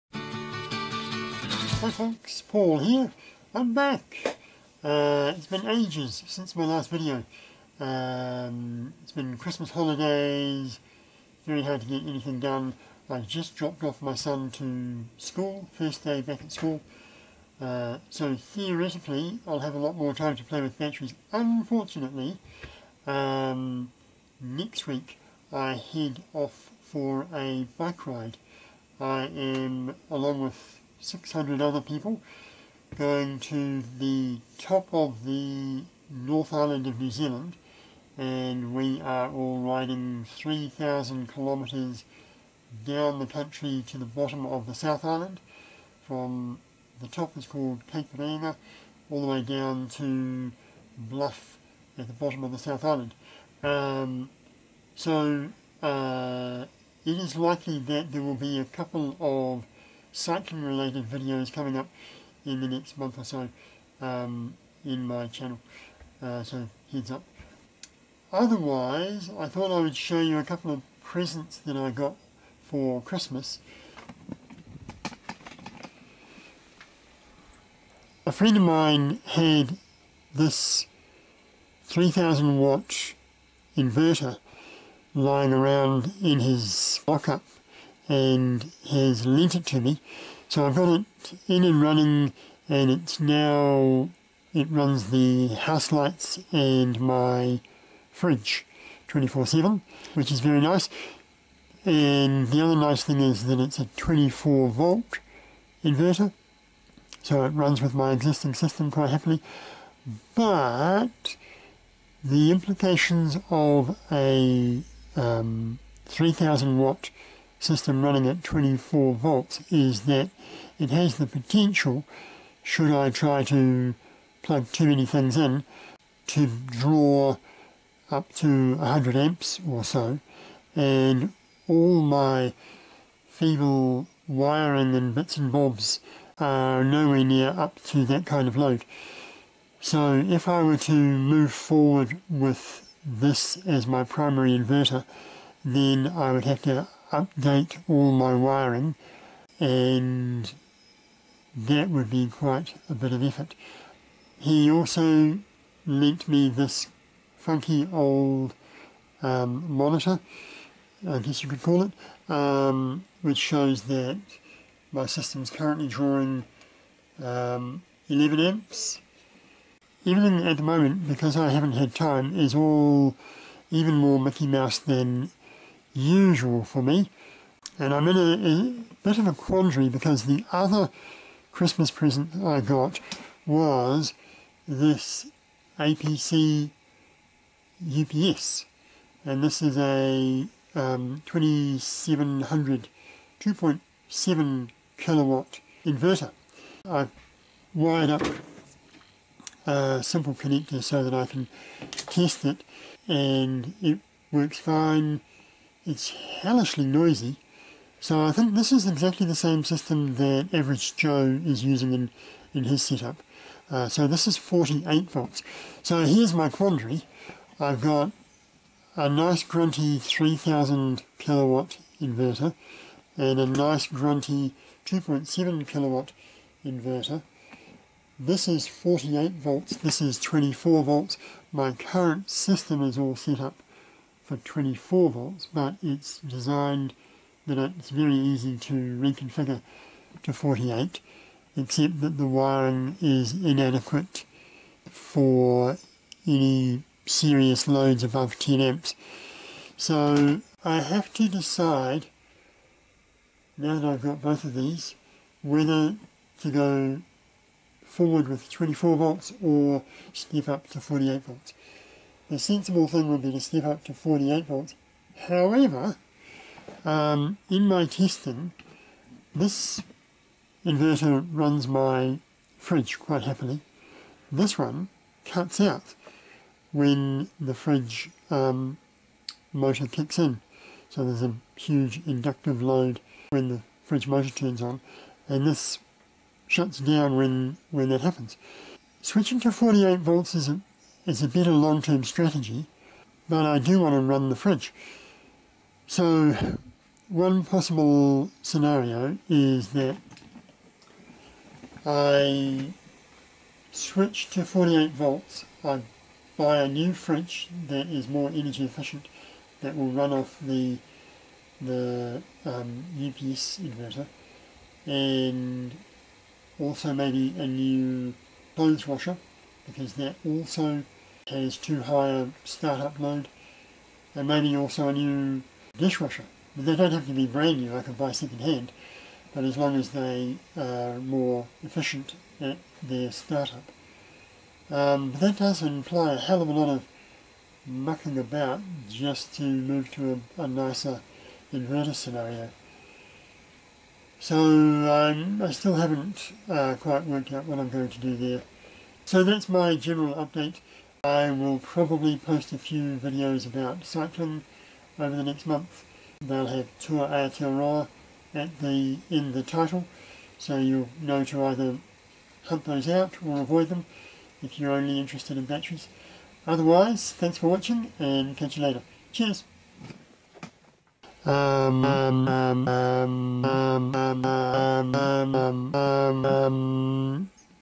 Laptop Powerwall 129 - Too many inverters
Suddenly I have too many inverters; a 24 Volt 3000 Watt one and a 48 Volt 2700 Watt UPS inverter (both pure sine wave). Should I stick with 24 Volts or make the jump to 48 Volts?

Plus on 10 Feb 2018 I start cycling the 3000 km Tour Aotearoa from the top to the bottom of New Zealand - so there may be some cycling related vids coming up.